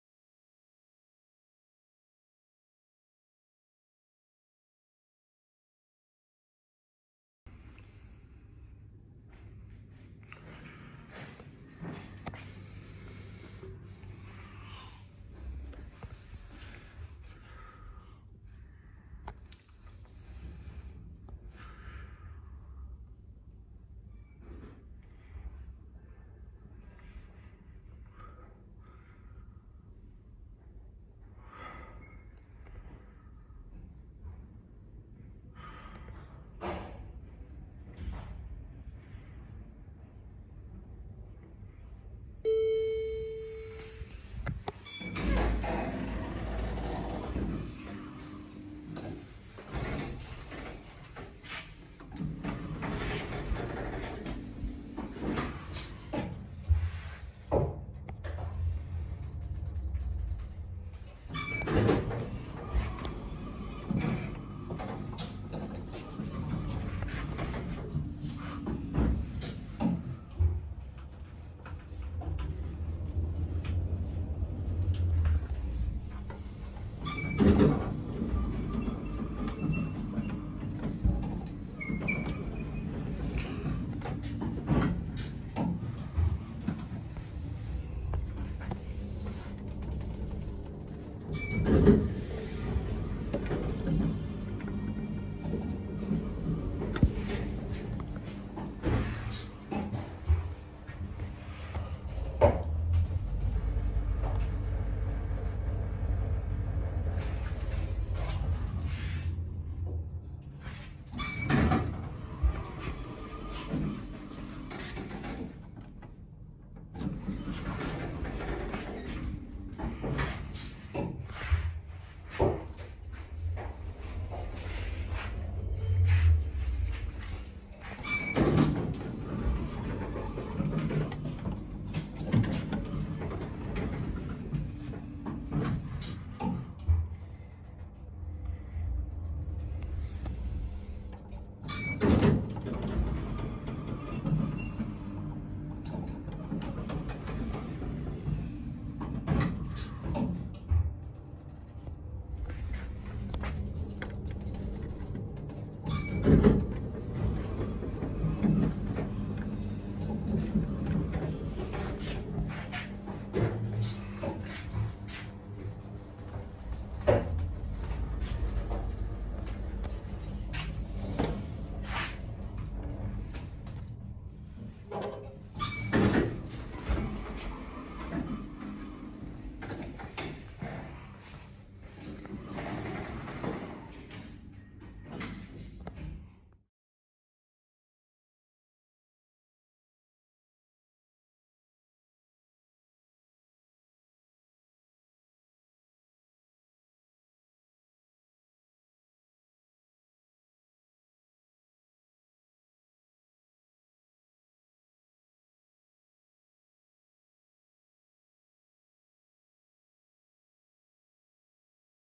[RT] 1998 Schindler traction elevator @ Azrieli Center, Tel Aviv, Israel.
Comments are disabled due to safety reason.

This used to serve the Bezeq offices before they moved to a new building in Holon. This is the only elevator that didn't get any anti-aesthetic braille sticker here. Making it the best "M-Series experience".

---Triangle Tower east carpark elevator---
Brand: Schindler
Type: Traction
Capacity: 1250kg / 16 Persons
Fixtures: M-Series
Floors: 5(-4, -3, -2, -1, *0*)